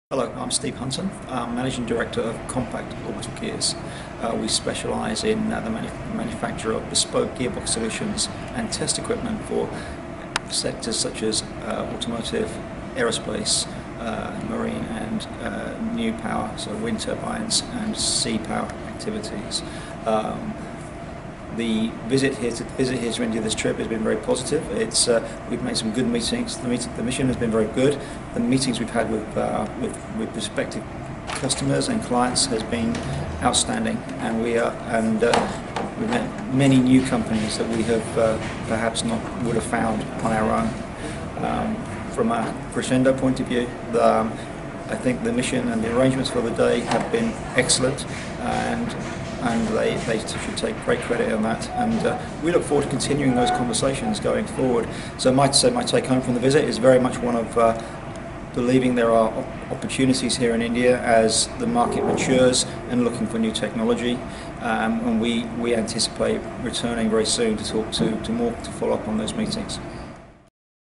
Compact Orbital Gears UK speaking about Crescendo Worldwide
Compact Orbital Gears UK - Managing Director Mr. Stephen Hunton, speaking about Crescendo Worldwide.